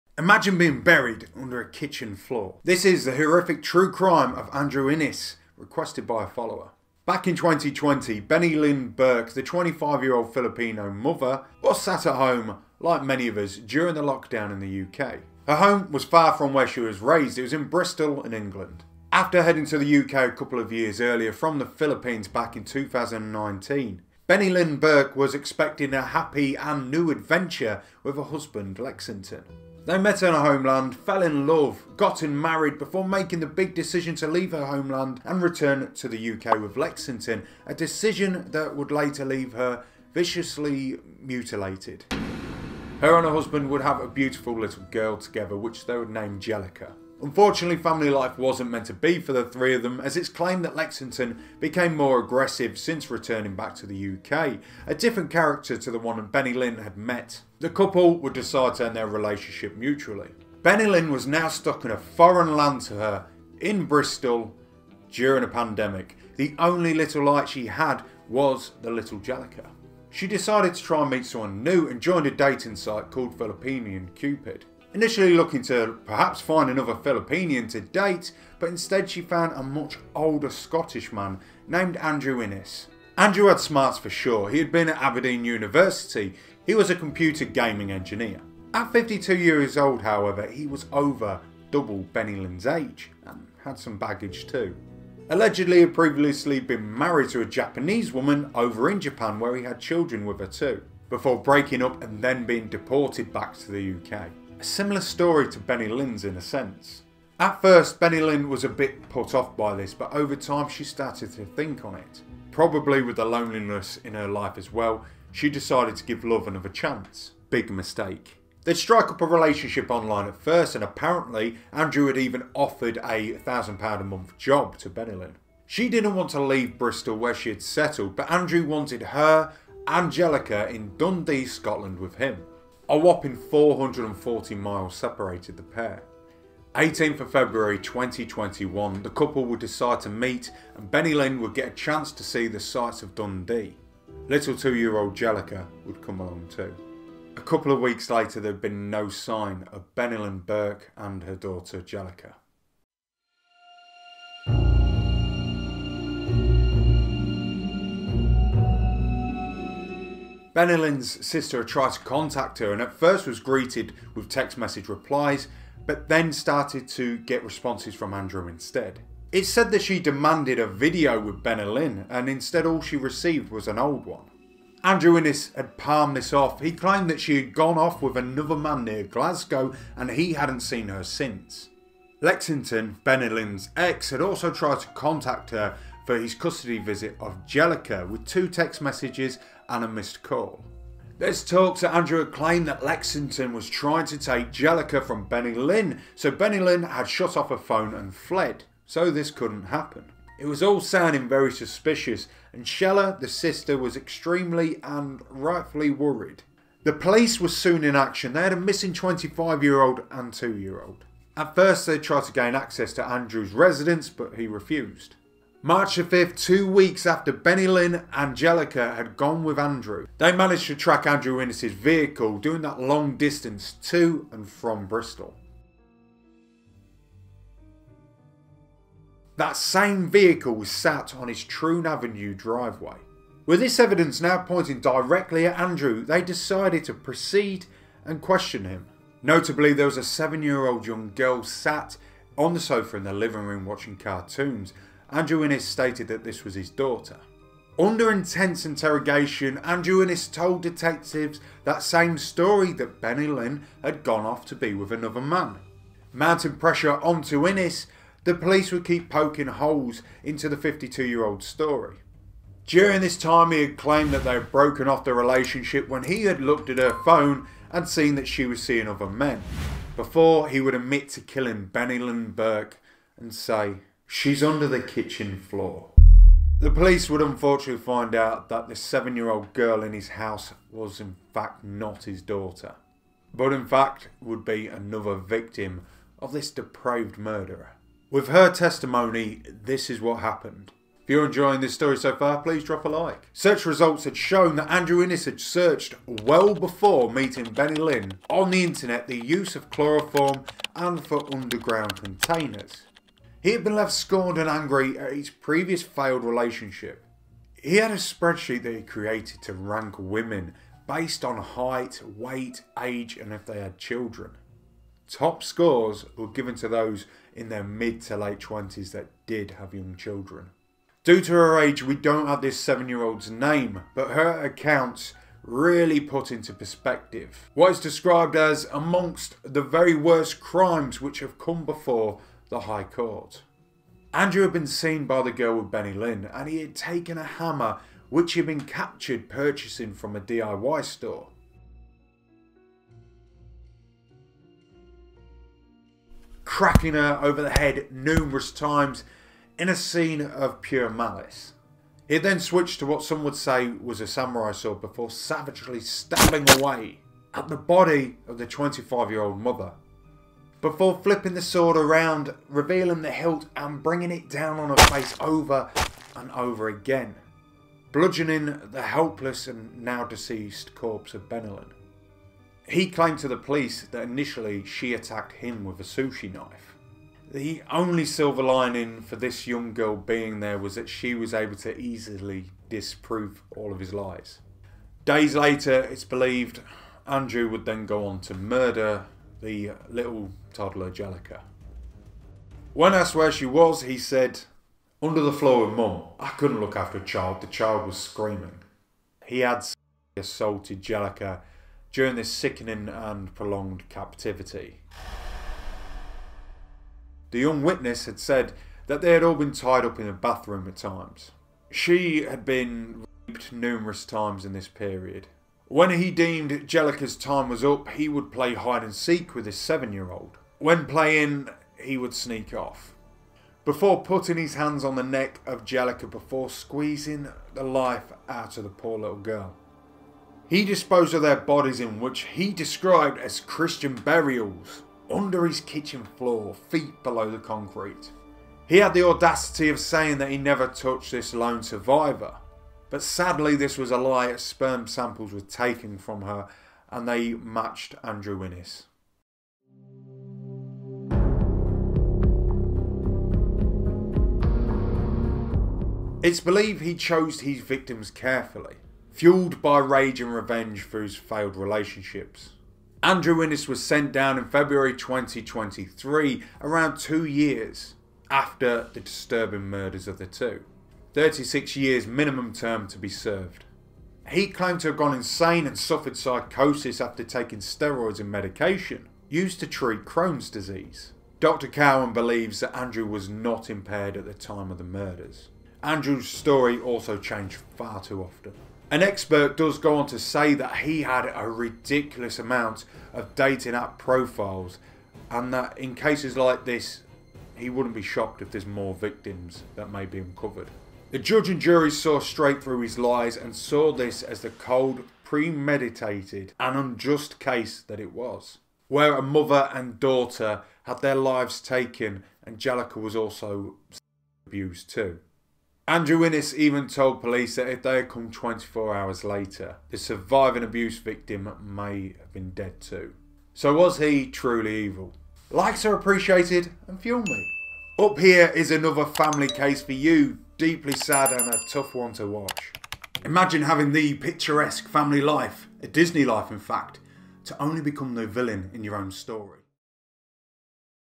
BODIES in the KITCHEN! Andrew Innes case [True Crime]
This as horrific true crime as you'll get. The true story of Andrew Innes, Bennylyn and Jellica Burke. The Dundee murders. The malicious and unforgettable murders in this crime story are enough to haunt anyone's sleep. Luckily enough true crime community, this is a solved case.
What did you make of this heinous murder? Today's case is a UK true crime.
UK murders.

Bennylyn Burke met Andrew Innes through Filipino Cupid, a dating site. They would date before Bennylyn and Jellica Burke would go missing leaving Andrew Innes as the prime suspect in this harrowing true crime case. Dubbed the Dundee Murders.

Thoughts? Was the killer Truly Evil?
I'm Ben, this is Truly Evil.